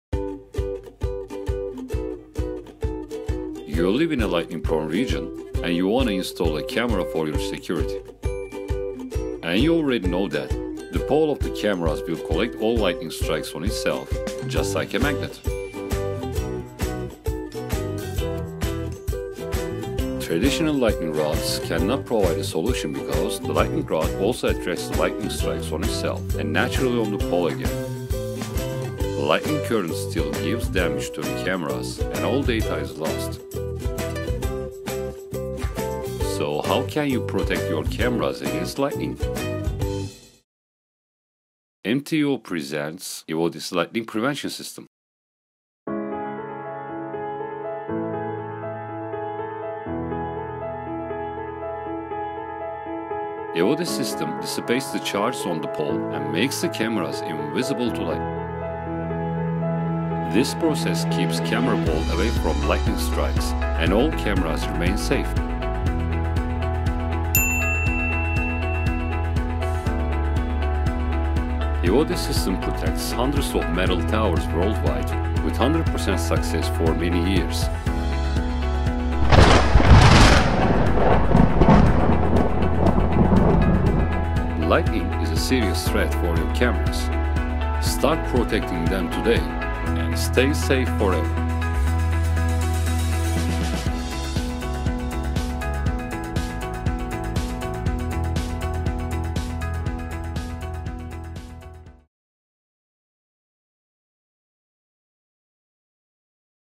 Proteção Contra Raios para Câmeras de Vigilância CFTV // Não há Mais Raios nas Câmeras
Os relâmpagos são uma séria ameaça para câmeras de CFTV e os para-raios tradicionais não podem proteger as câmeras contra raios.

O Sistema EvoDis é a ÚNICA solução de proteção contra raios que mantém o raio longe das câmeras de CFTV.

Sem mais raios, sem mais danos nas câmeras, sem mais perda de serviço.